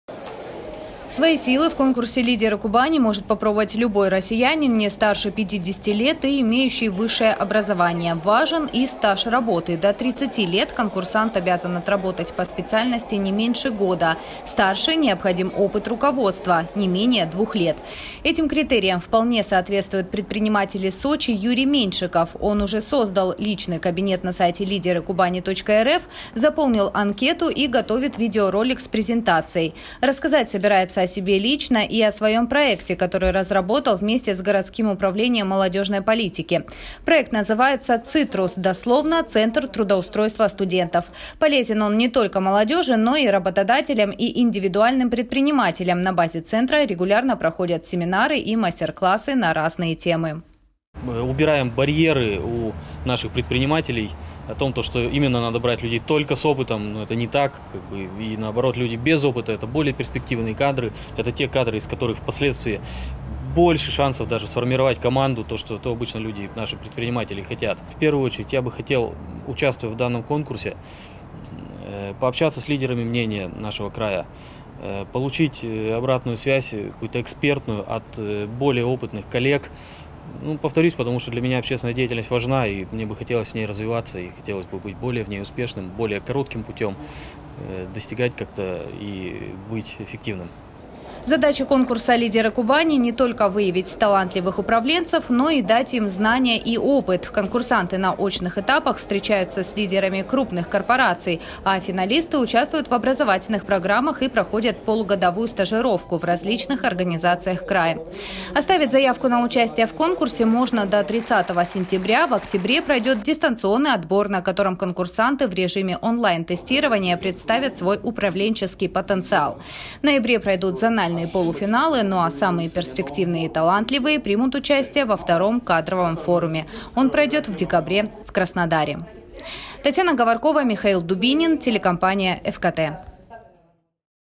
Сочинцы еще могут подать заявки на участие в конкурсе «Лидеры Кубани»
В Краснодарском крае стартовал кадровый конкурс «Лидеры Кубани. Движение вверх». Решение о его проведении было принято губернатором в прошлом году. Цель конкурса – выявить перспективных менеджеров и руководителей. Регистрация участников началась 1 сентября. Немало заявок отправлено из Сочи.

Свои силы в конкурсе «Лидеры Кубани» может попробовать любой россиянин не старше 50 лет и имеющий высшее образование. Важен и стаж работы. До 30 лет конкурсант обязан отработать по специальности не меньше года. Старше – необходим опыт руководства не менее двух лет.

Этим критериям вполне соответствует предприниматель из Сочи Юрий Меньшиков. Он уже создал личный кабинет на сайте лидерыкубани.рф, заполнил анкету и готовит видеоролик с презентацией. Рассказать собирается о себе лично и о своем проекте, который разработал вместе с городским управлением молодежной политики. Проект называется «ЦиТРУС» — Центр трудоустройства студентов. Полезен не только молодежи, но и работодателям, и индивидуальным предпринимателям. На базе центра регулярно проходят семинары и мастер-классы на разные темы.